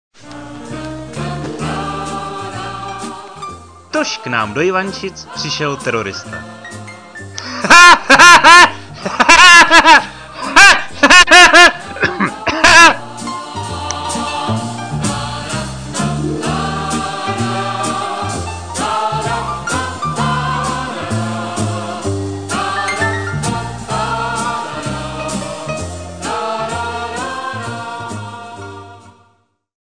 Vladimír Menšík - Al Kakajdá
Vladimír Menšík vypravuje historku, jak se u něj na chalupě stavil soused po návratu z dovolené v Sýrii a jaký suvenýr mu přivezl.